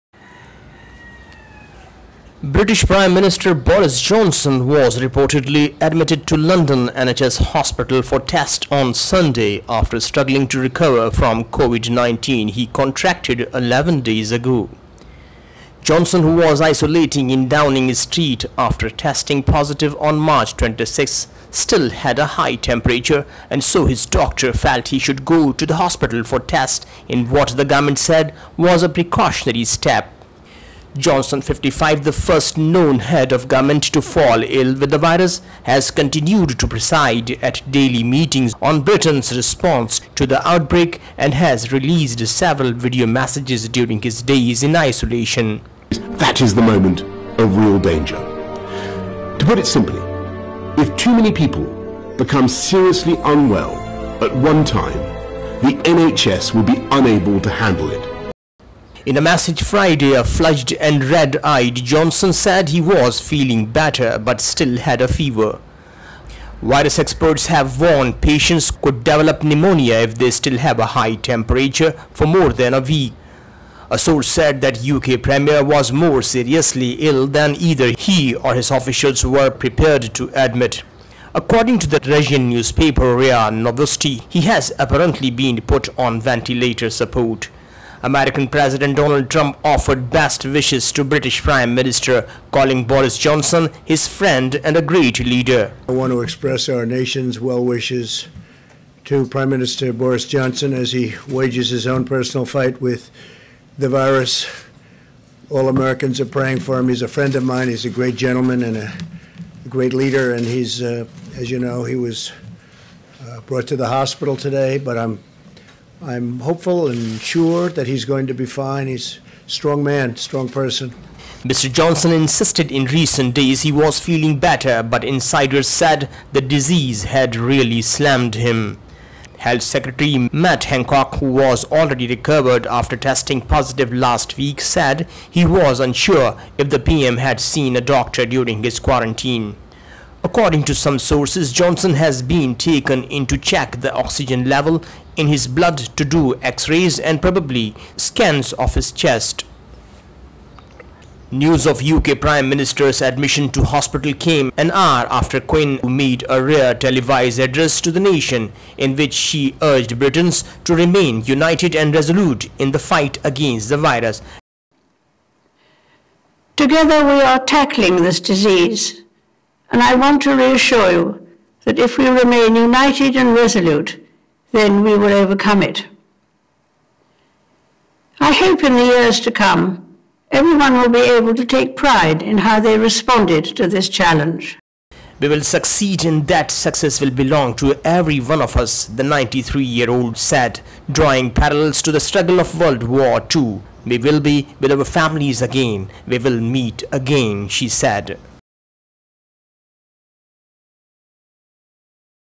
Boris Johnson admitted to hospital
Boris Johnson put on ventilator?
British Prime Minister Boris Johnson was admitted to London NHS hospital on Sunday after struggling to recover from COVID-19. According to some sources UK PM has been put on ventilator support.